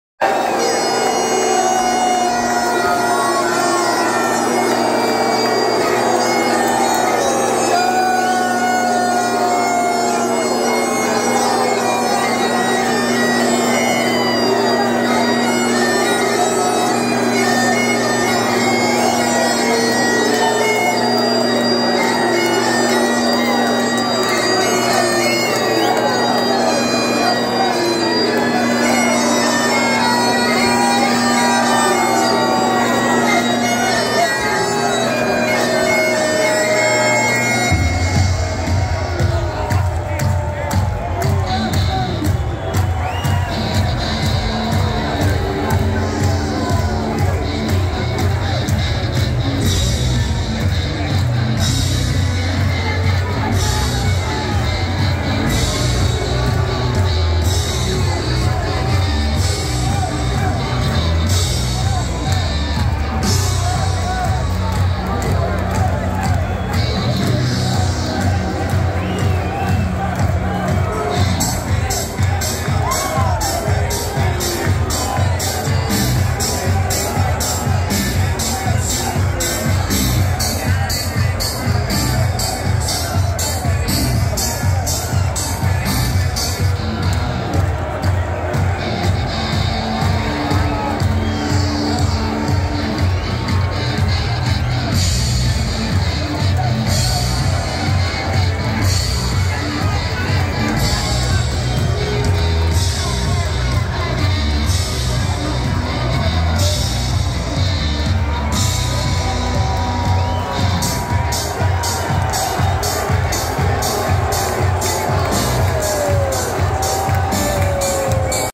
Josh Taylor Ring Walk At MSG NY
AndNew Junior Welterweight @teofimolopez 🔥
Congrats to Lopez on dominate performance over @joshtaylorbox1 in title fight in MSG.
The judges had it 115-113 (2x) and 117-111 for Lopez.
Thoughts on scorecards by judges?
Who is next for Lopez?
Like 👍🏽, Comment 🗯, Tag 🥊, Follow 🏃‍♂️